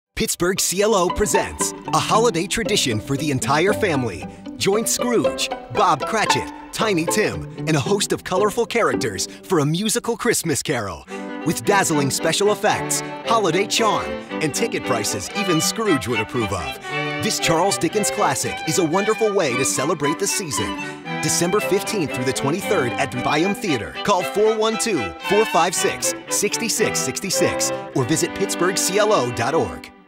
A MUSICAL CHRISTMAS CAROL | Dec 15-23
A HOLIDAY TRADITION FOR THE ENTIRE FAMILY!
We've taken the spirit of the season, wrapped it in your favorite yuletide melodies, and tied it up with all the holiday magic and wonder of your childhood. Join Scrooge, Bob Cratchit, Tiny Tim, and a host of colorful characters for A MUSICAL CHRISTMAS CAROL. With dazzling special effects, holiday charm, and ticket prices even Scrooge would approve of, this Charles Dickens classic is a wonderful way to celebrate the season.